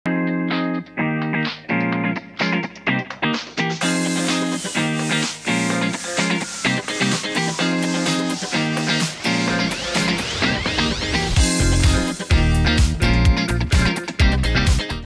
Great value broadband, with even greater rewards | MyRepublic MyRewards
With hundreds of retail partners, you can get cashback on your broadband bill while you shop.